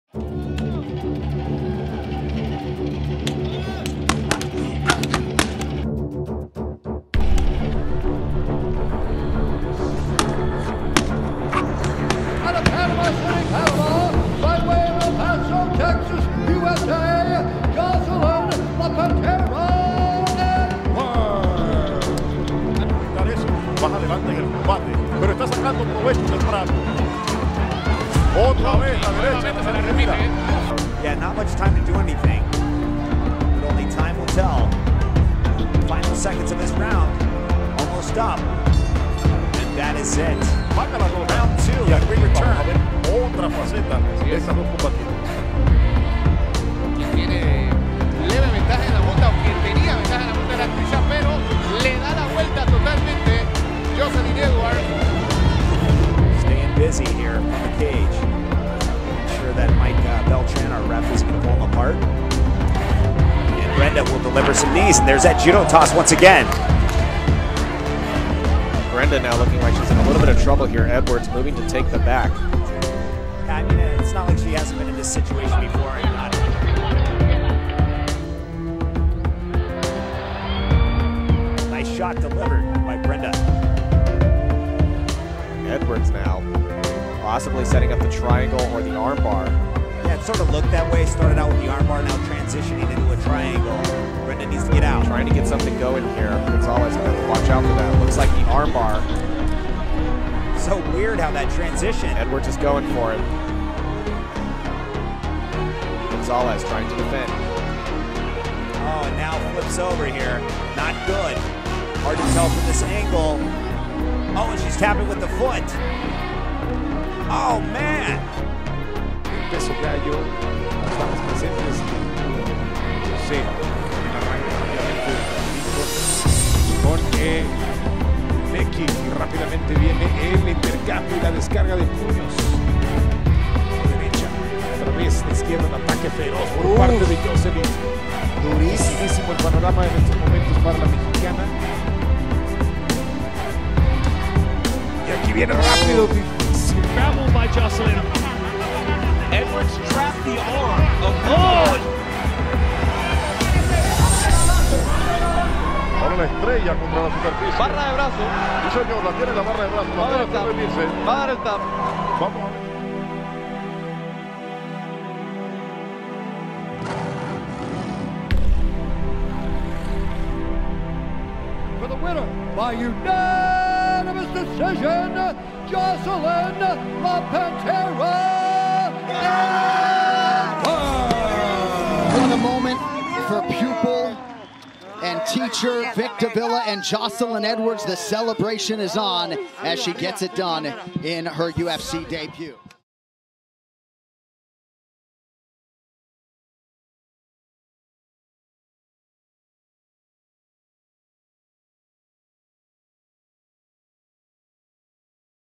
Joselyne Edwards MMA Highlights - La Pantera
Joselyne Edwards the first female athlete from Panama fighting for UFC. Joselyn has started her martial arts career training Ju-Jitsu Fighting System (JJIF) in which she has achieved a lot of trophies, after switching to MMA she spectacularly won a lot of fights and finally became the first female athlete from Panama with a UFC debut.

Joselyne Edwards la primera atleta femenina de Panamá que lucha por UFC. Joselyn ha comenzado su carrera de artes marciales entrenando Ju-Jitsu Fighting System (JJIF) en el que ha logrado muchos trofeos, luego de cambiarse a MMA ganó espectacularmente muchas peleas y finalmente se convirtió en la primera atleta femenina de Panamá con un debut en UFC.